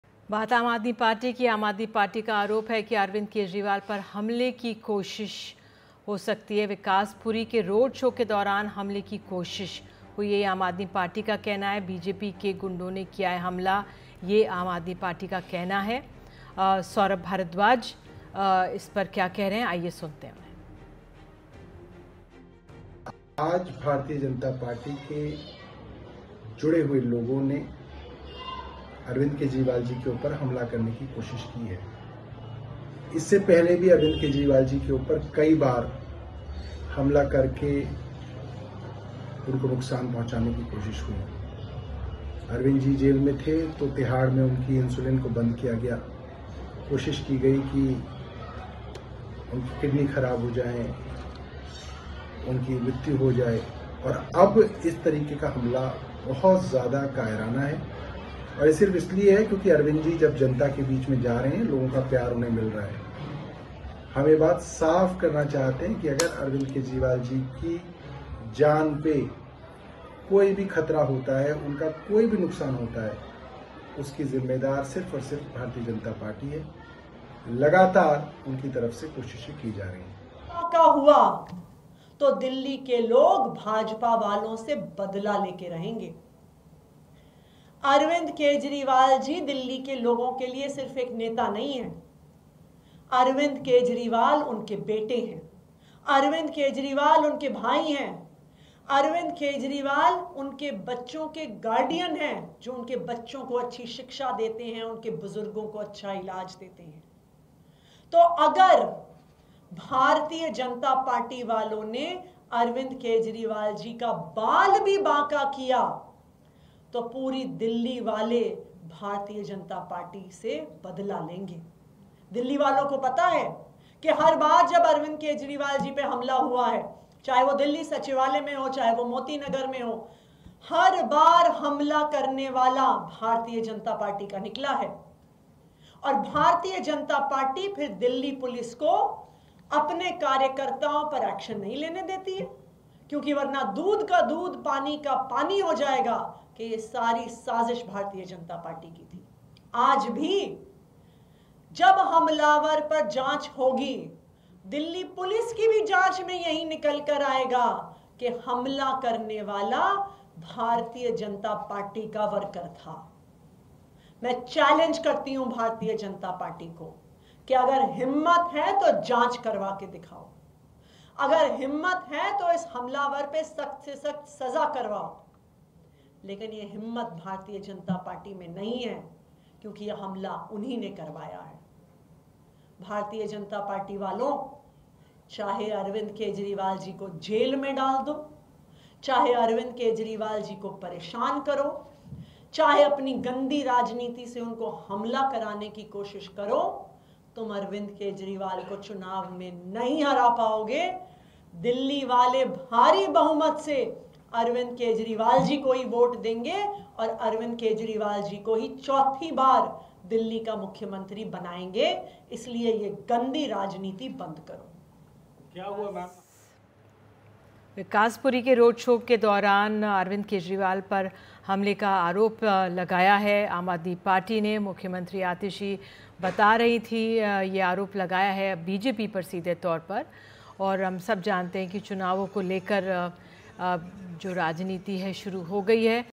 Arvind Kejriwal को कुछ हुआ तो BJP जिम्मेदार, Delhi वाले लेंगे बदला: CM Atishi | Breaking News
Atishi On BJP: दिल्ली के पूर्व मुख्यमंत्री अरविंद केजरीवाल पर पदयात्रा के दौरान हमले का मामला सामने आया है. आम आदमी पार्टी ने केजरीवाल पर हमले का आरोप BJP पर लगाया है. वहीं, दिल्ली की CM आतिशी ने शुक्रवार को कहा कि अगर अरविंद केजरीवाल को कोई नुकसान पहुंचा, तो इसकी जिम्मेदारा BJP होगी. केजरीवाल को कुछ हुआ, तो दिल्ली की जनता BJP से बदला लेगी.